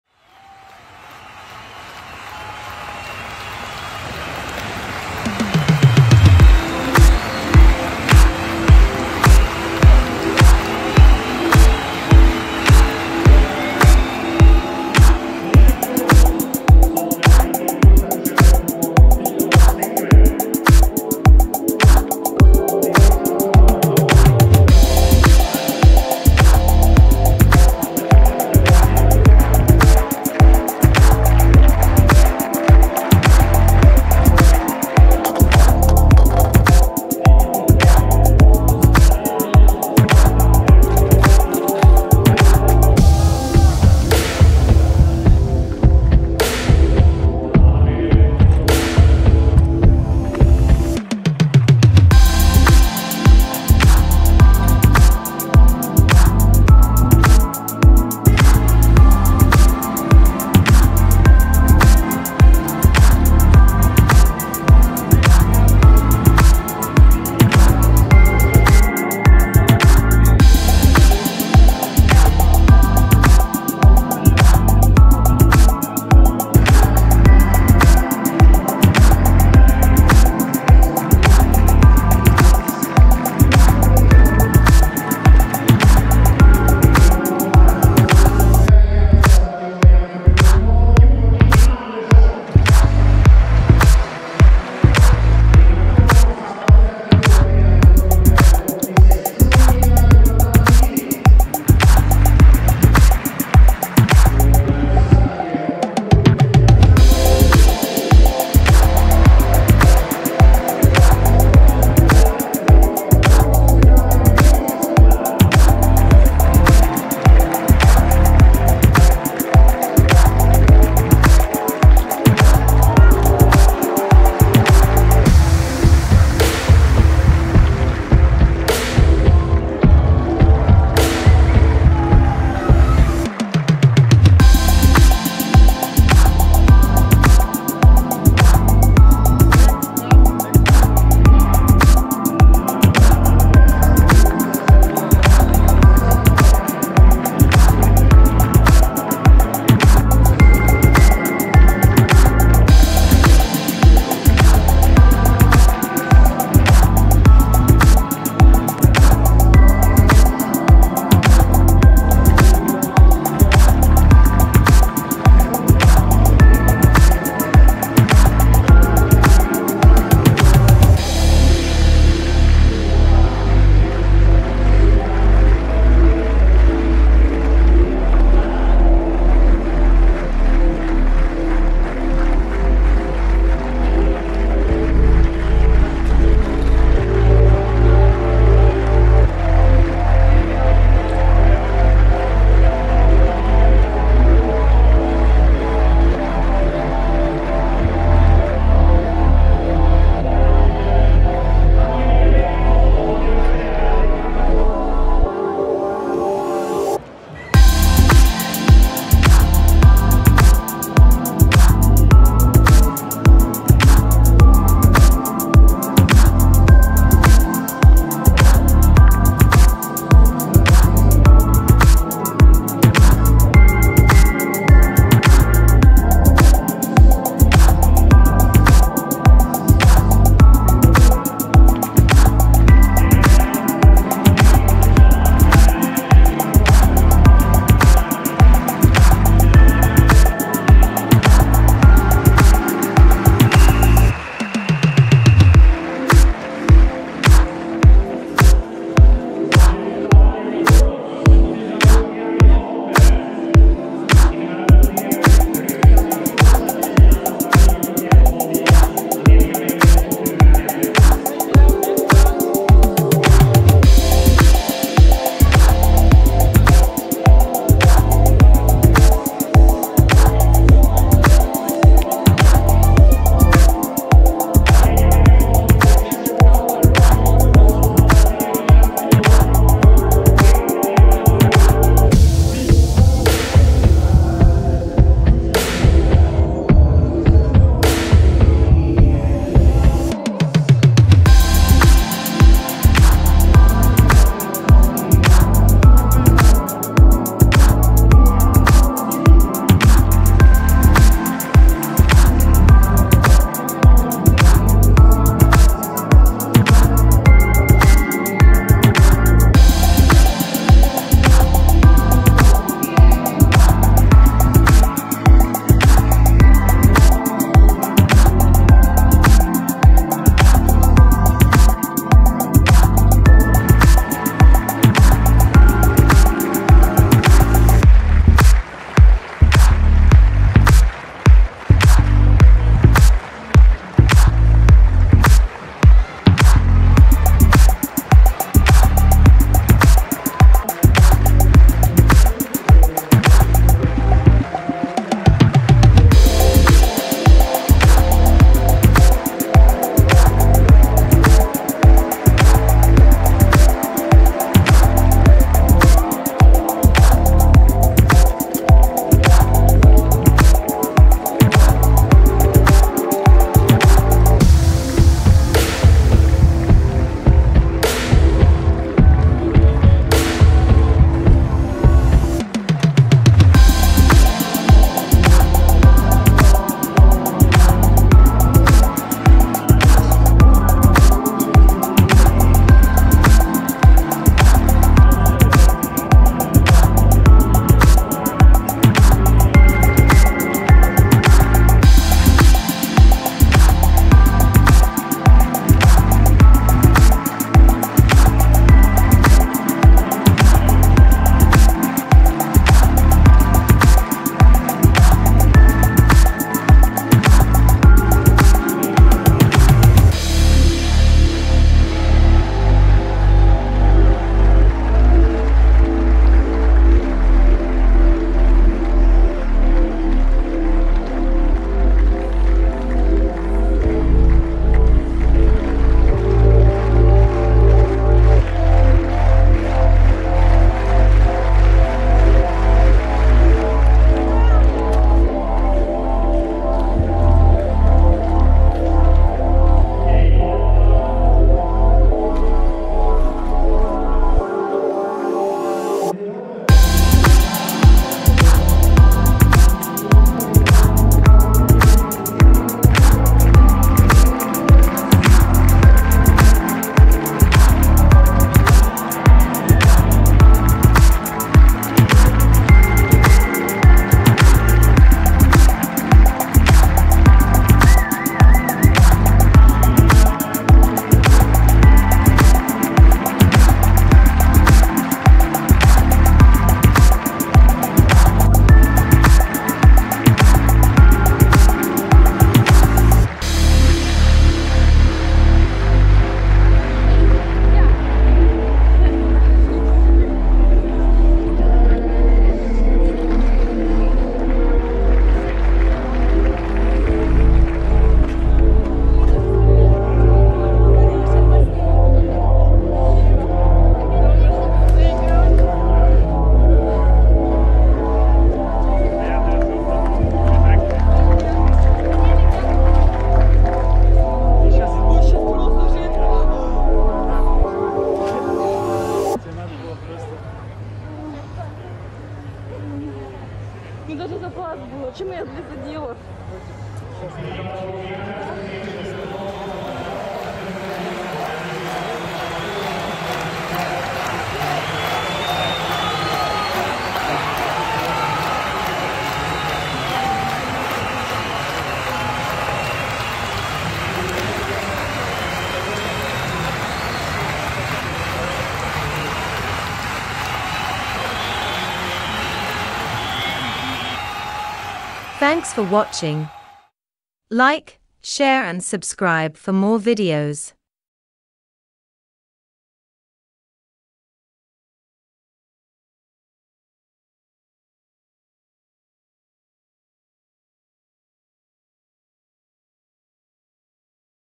Polina Knoroz - Women's Pole Vault
Athlete: Polina Knoroz
Country: Russia
Category: Pole Vault
Event: Stockholm 2021 - Diamond League